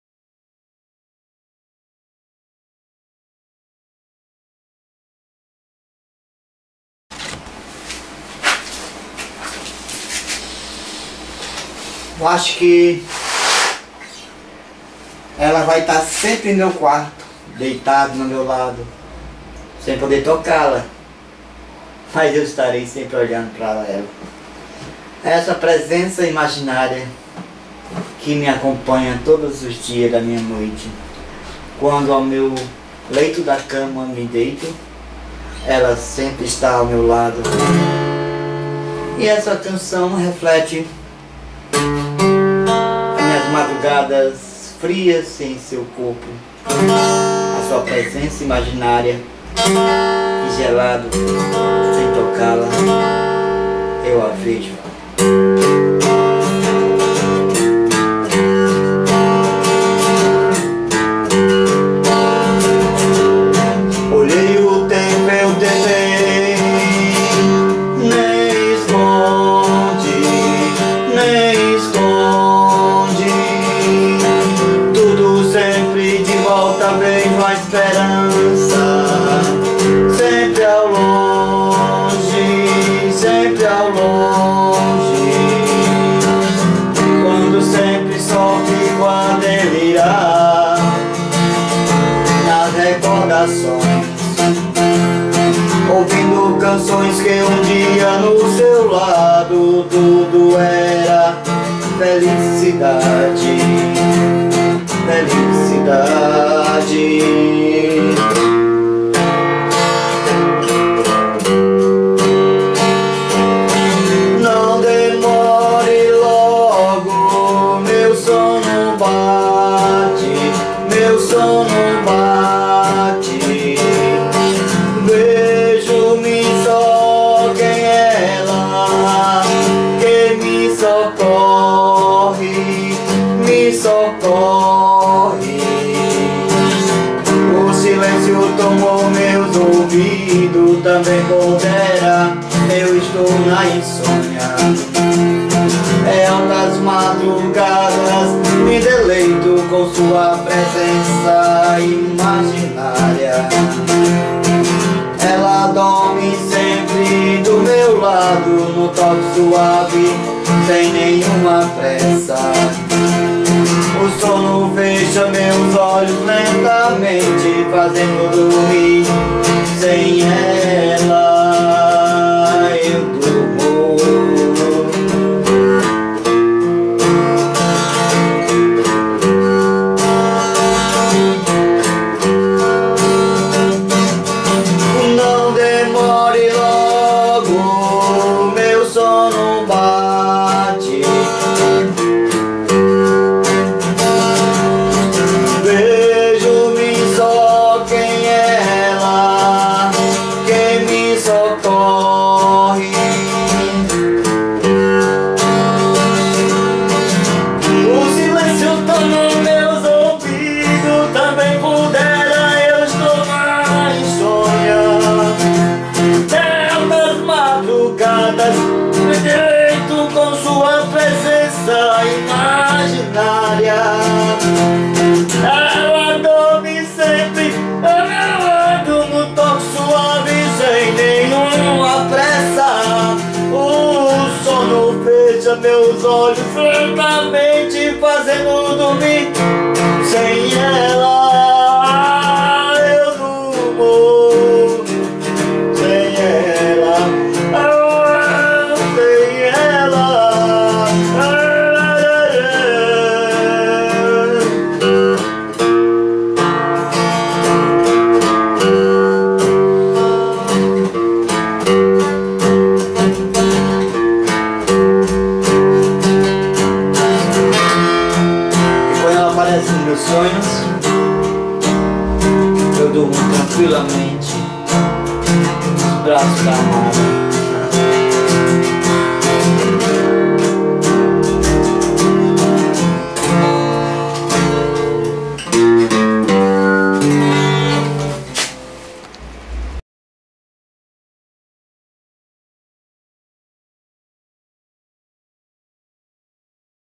PRESENÇA IMAGINÁRIA.Ley
0.232 PRESENÇA IMAGINÁRIA.Ley

Olhei o tempo.
Tentei e nem esconde.
Tudo sempre de volta.
Vejo uma esperança
Sempre ao longe

Quando sempre só
Fico a delirar!
Recordações:
Ouvindo canções
Que um dia
Em seu lado
Tudo era felicidade

Não demore
Logo o sono bate
Vejo-me só
Quem mim socorre?

O silencio tomou os meus ouvidos
Também pudera! Estou na insônia
É altas horas da madrugada
Deleito-me com a sua presença, imaginária!

Ela dorme sempre ao meu lado
Num toque suave sem nem uma pressa
O sono fecha os meus olhos lentamente

Fazendo-me dormir sem ela